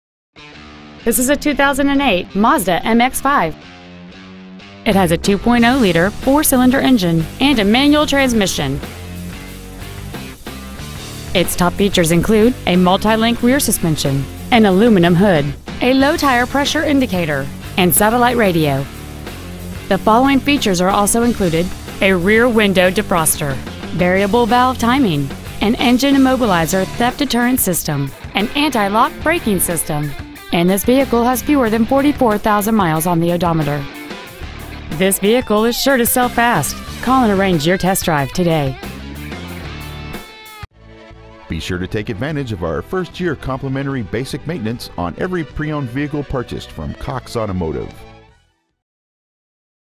2008 Mazda MX-5 Sarasota FL
Year : 2008
 Make : Mazda
 Model : MX-5
 Engine : gas i4 2.0l/122
 Trans . : manual
 Exterior : black
 Miles : 43,011
 Interior : black

 Used 2008 Mazda MX-5 Bradenton FL